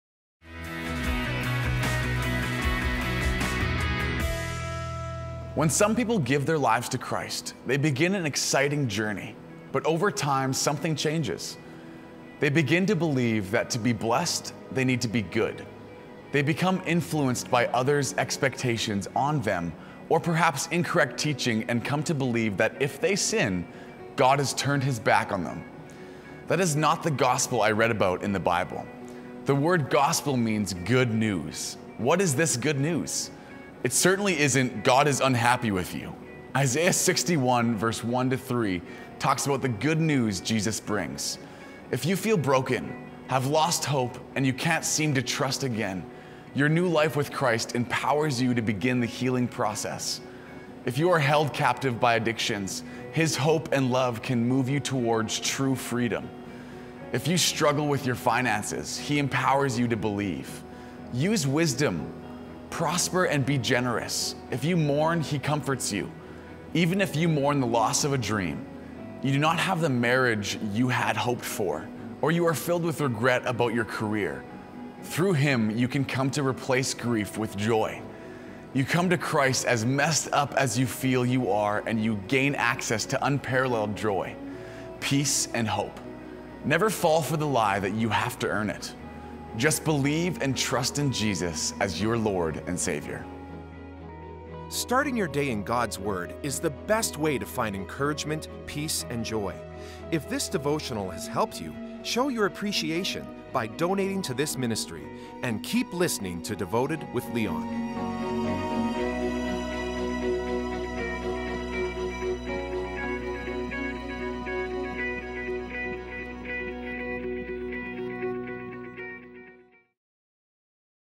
Devoted: What’s so Good About the Good News? [Isaiah 61:1-3]
Today's Topic - Come to Christ as messed up as you feel you are and you can gain access to unparalleled joy, peace and hope.

Today's Verse - Isaiah 61:1-3

Do you have any prayer requests?